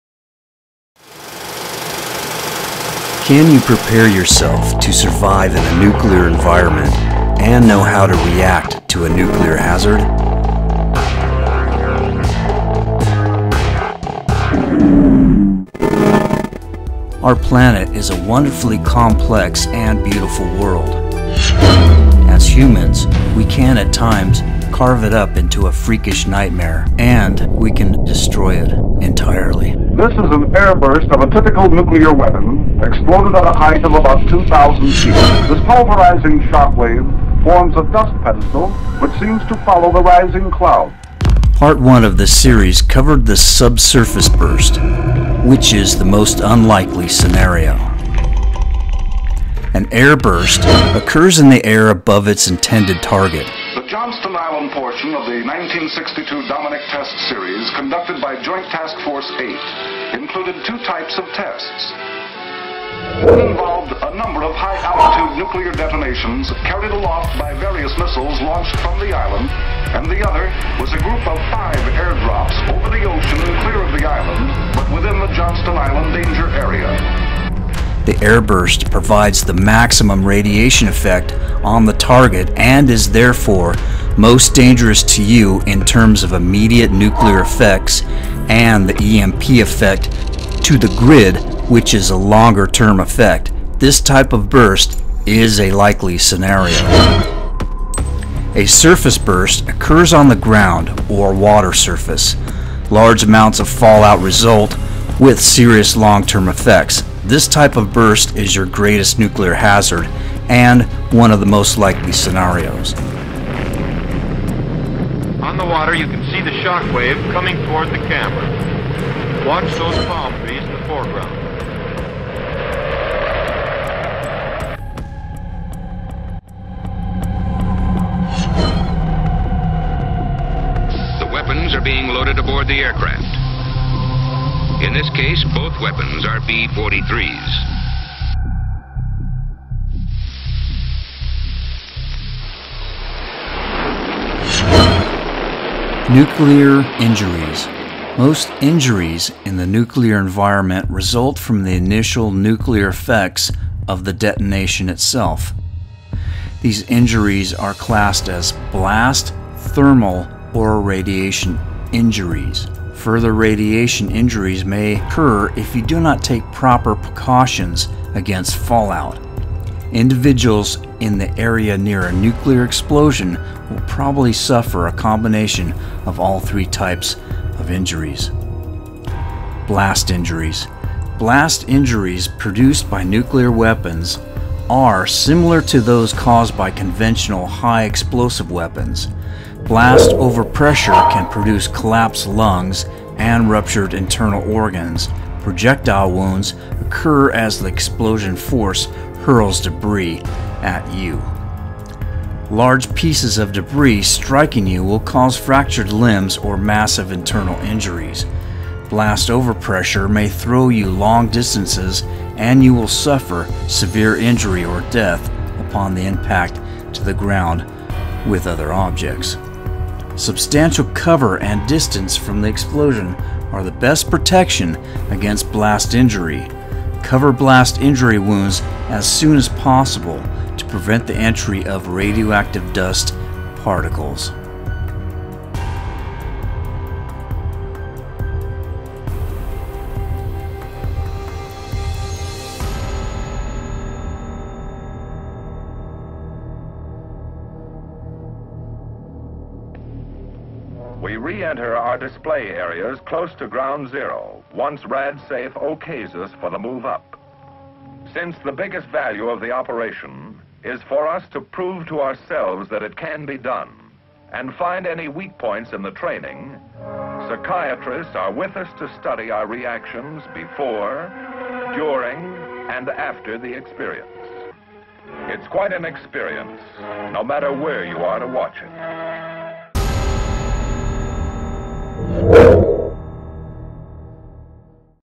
Nuclear detonation (part two)
Surviving a Nuclear detonation (Part Two) I edited a misspelled word from the original upload, it would have bugged me to no end.

Surviving a nuclear detonation, Nuke Map simulation.